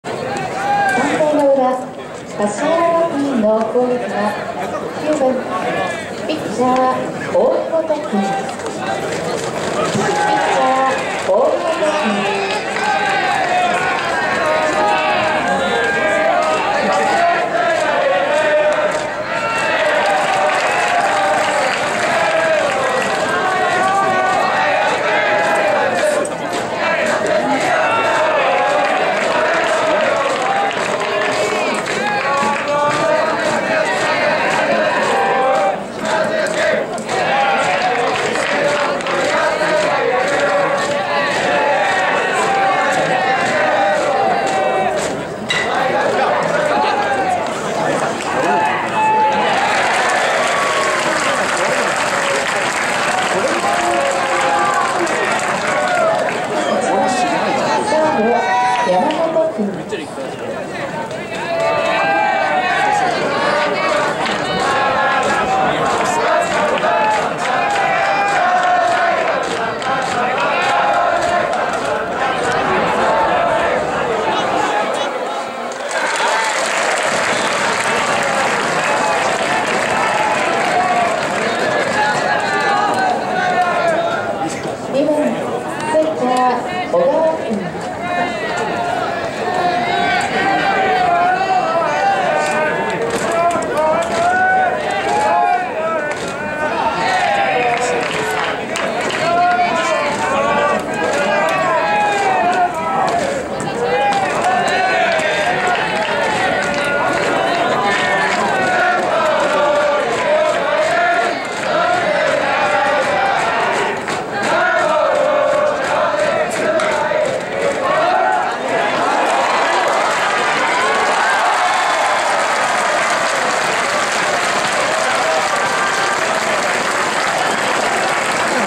2015/5/10 高田商vs橿原学院 ３回裏 橿原学院
奈良県高校野球春季大会
決勝＠佐藤薬品スタジアム
高田商vs橿原学院
３回裏
橿原学院
⑨扇本①山本②小川
高田商５－０橿原学院